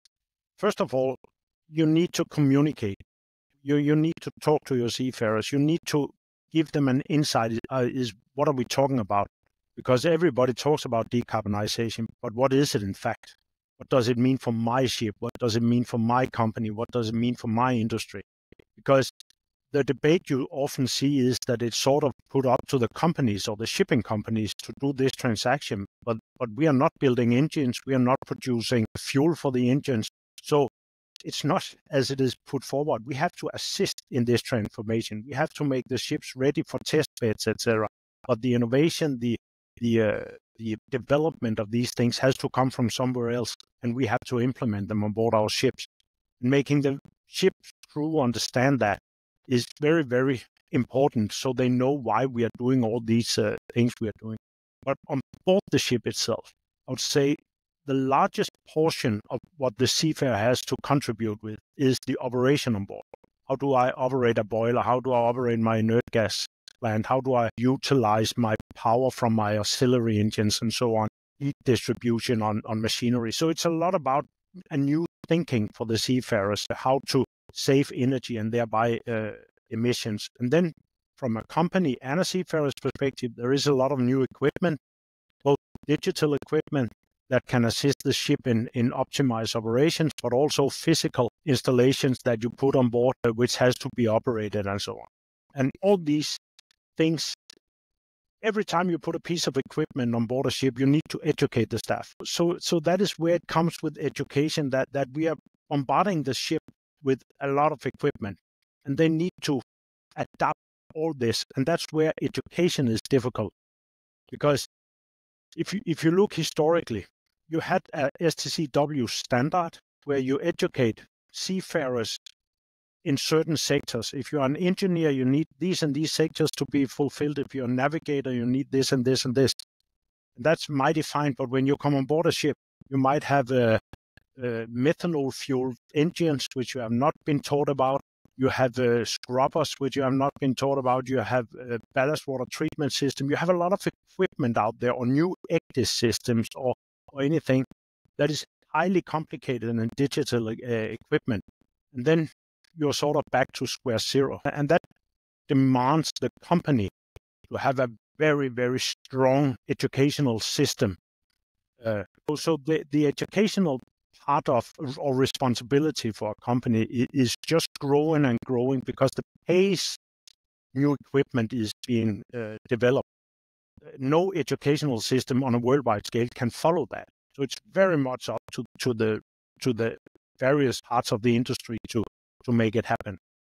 Decarbonization And The Seafarers
Is your maritime business prepared for the future? Ralph Juhl of Hafnia shares valuable insights on the shifts already underway—are you ready to stay ahead?

The maritime industry is experiencing significant changes, from decarbonization to digitalization. For many businesses, keeping up with these advancements can be challenging, but it's essential for future success.

As your competitors adopt new technologies and strategies, staying informed and proactive is key. By embracing these changes early, your business can remain competitive in the evolving landscape.

In my latest podcast episode with Ralph Juhl, we discuss the future of maritime, the role of digitalization, and strategies to help businesses thrive as we approach the future.

Check out this short clip!